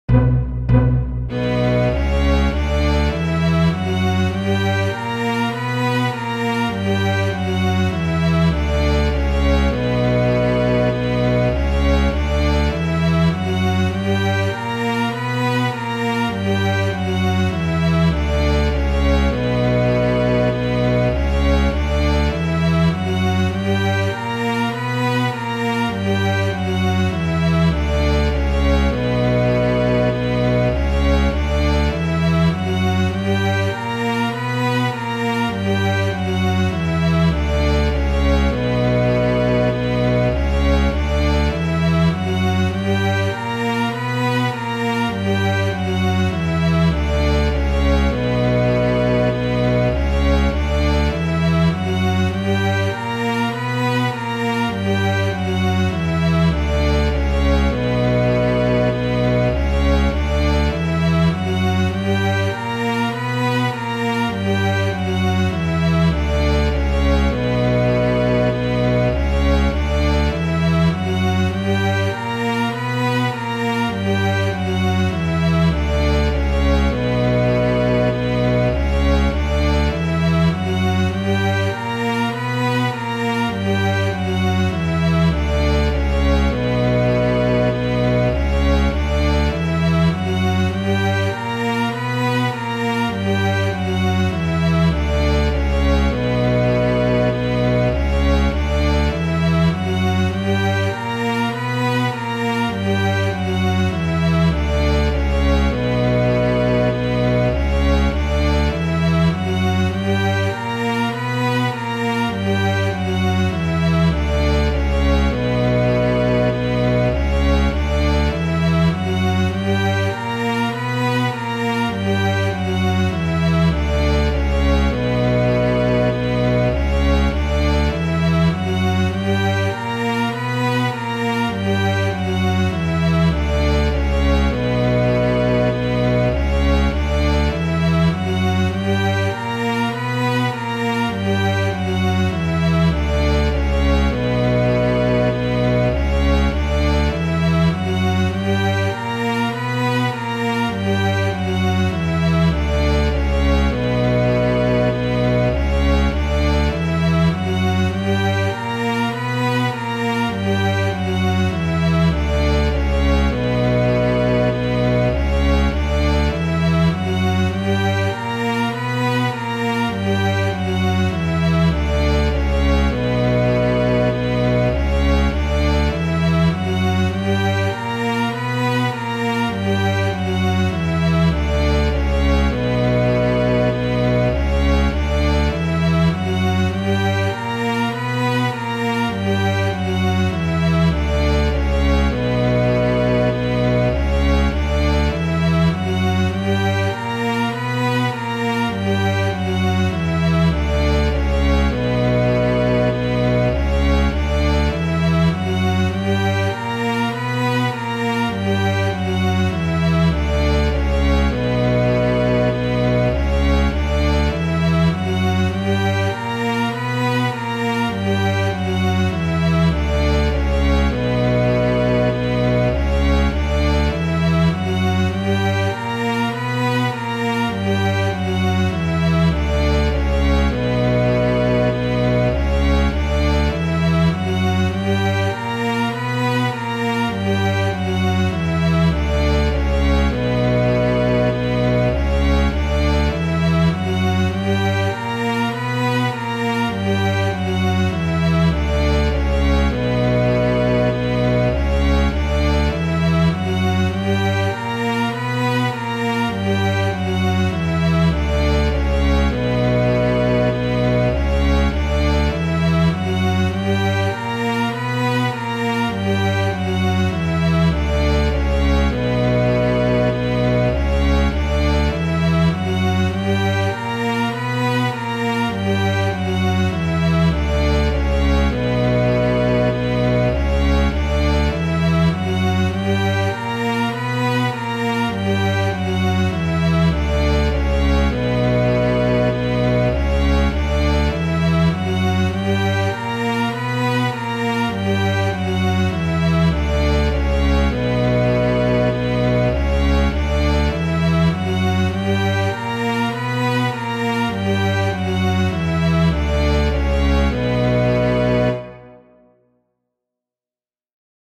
B Harmonic Minor Scale One Octave at 100bpm Backing Track BnW
Play or sing along. This track features double bass, cello, viola and violin playing B Harmonic Minor Scale One Octave at 100bpm. Sheet music is provided on G treble clef, C alto clef and F bass clef.